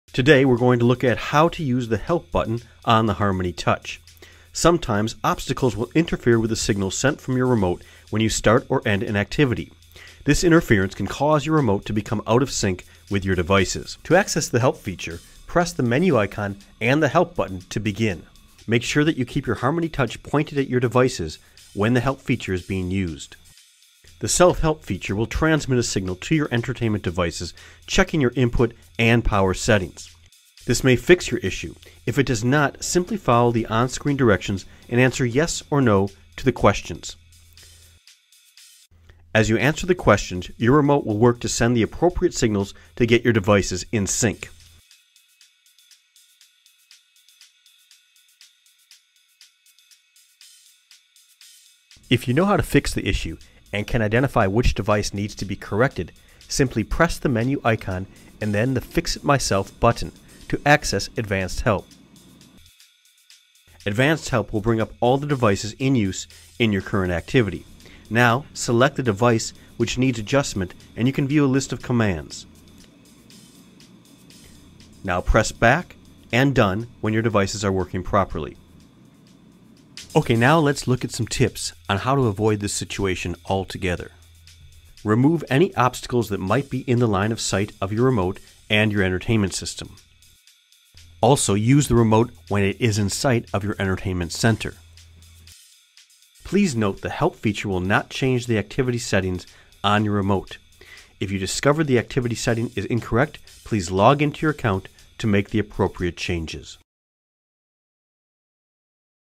Harmony Touch "Help" Feature
This video shows you how to correct problems with your devices being out of sync with your remote.  The "Help" feature allows you to do this quickly and easily.  Learn how to use this feature and get some helpful tips in this video.

Questions?